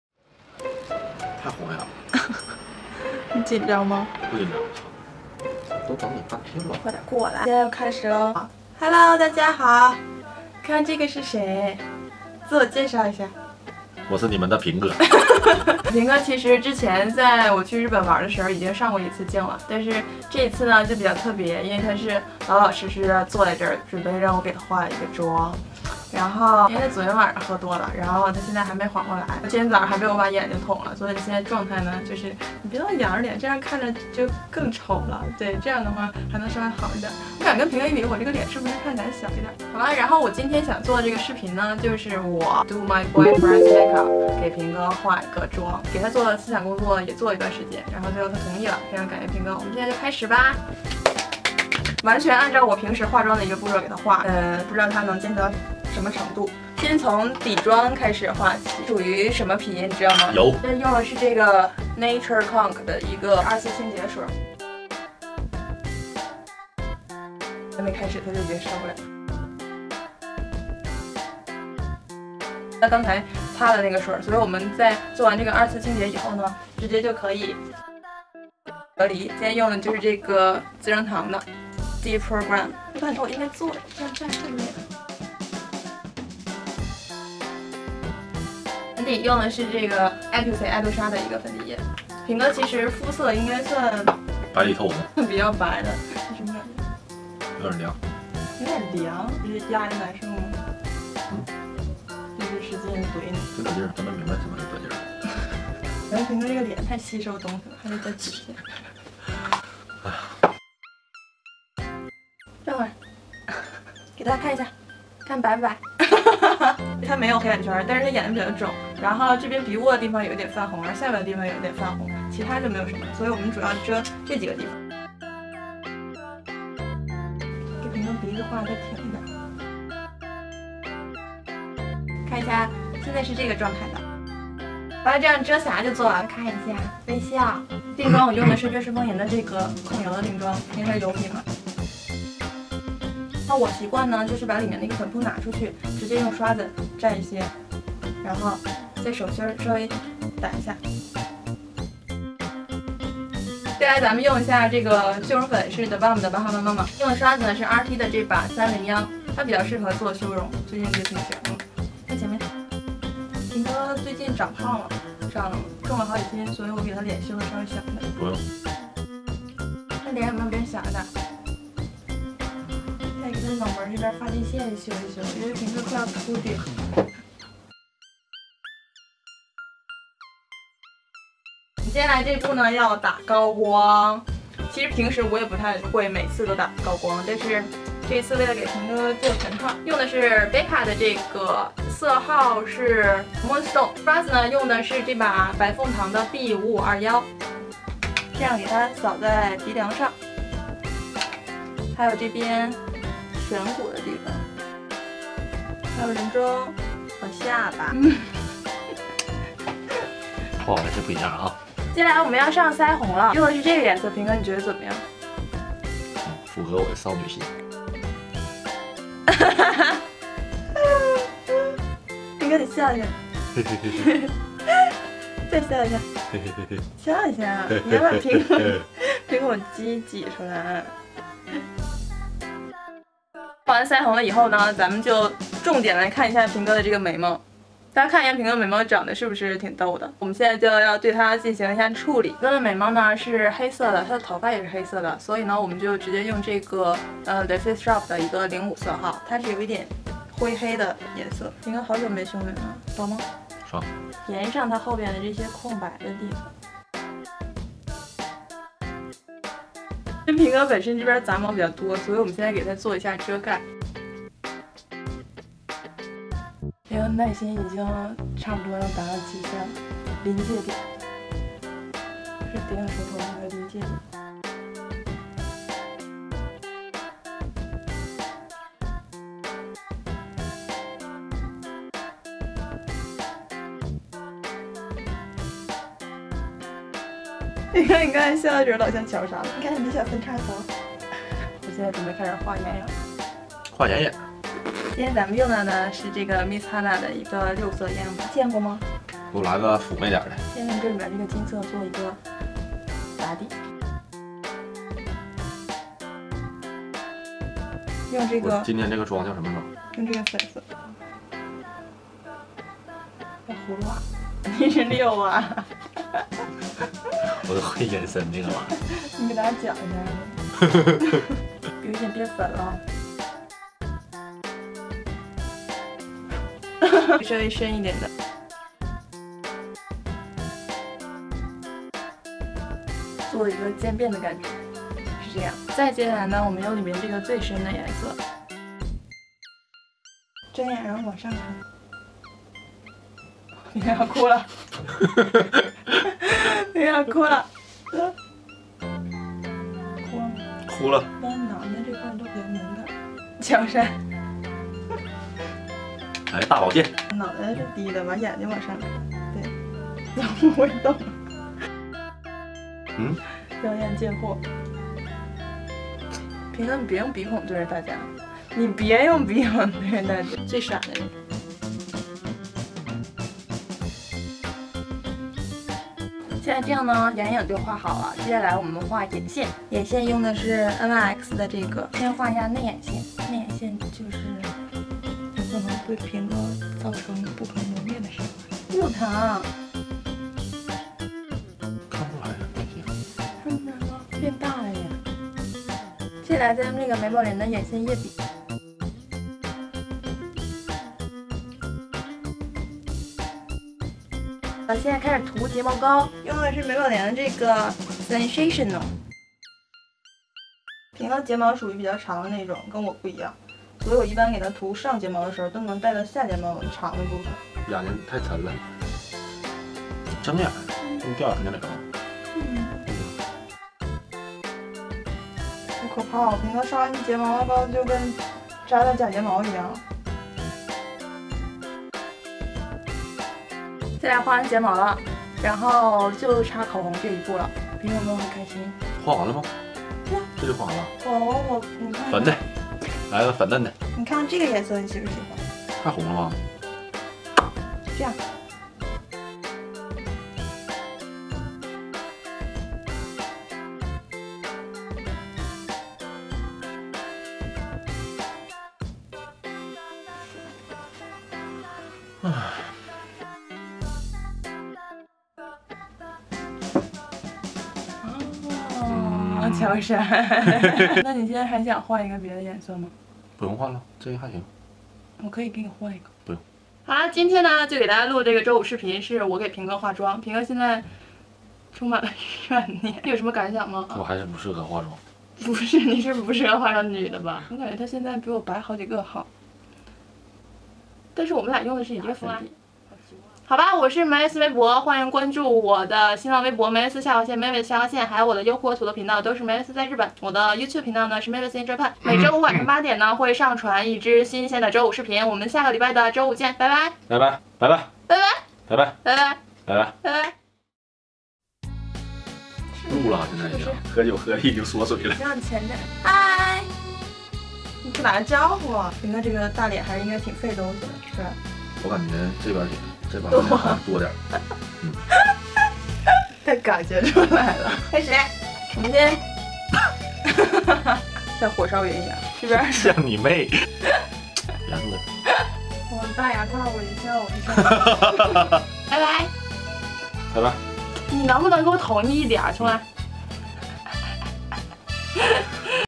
给男友化妆｜Girlfriend Does Boyfriend's Makeup｜Mavis
哈咯大家好我是梅博
这次的周五视频盛情邀请平哥给大家做一档搞笑节目
平哥跟我异地恋加同地恋转眼已经1+3年的时间，所以不管是默契还是傻劲都不言而喻了，没有虐狗的企图也没有晒幸福的资本，只是看YouTube上面这个话题蛮搞笑，刚好又有时间就做啦

希望能博大家一笑，我俩就知足啦

 男友妆使用产品：

1.Nature Conc清洁化妆水
2.Silcot 化妆棉
3.d program奶瓶隔离
4.艾杜纱 amino粉底液 N20
5.Miss Hana三色遮瑕膏
6.Innisfree悦诗风吟 控油散粉
7.The Balm 修容粉
8.Becca 高光粉
9.The Balm 腮红
10.The Face Shop 眉笔 ＃05
11.Miss Hana 六色眼影盘
12.NYX 眼线笔
13.Maybelline美宝莲 眼线液笔
14.美宝莲 Sensational 睫毛膏
15.Loreal欧莱雅 女神口红